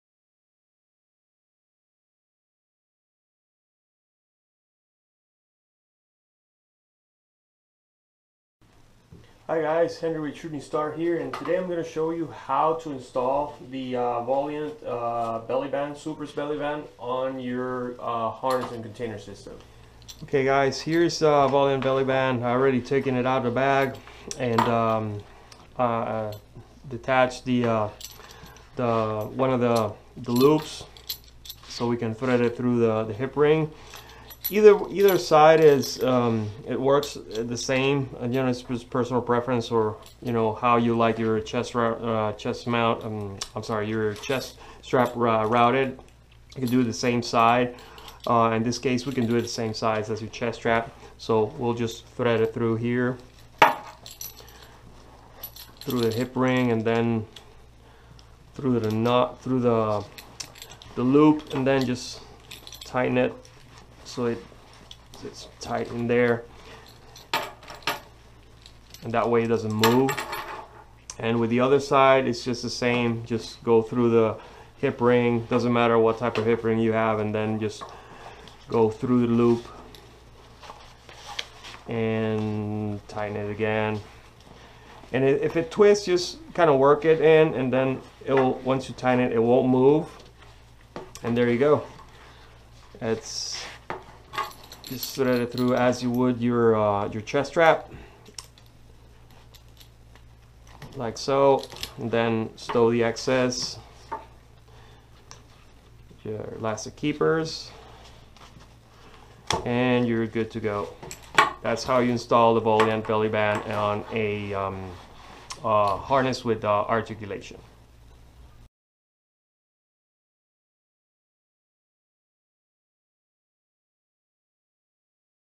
Installing Volitant Padded Adjustable Belly Band
Henry Kochen shows you how to properly install the Volitant Padded Adjustable Belly Band to your harness and container system.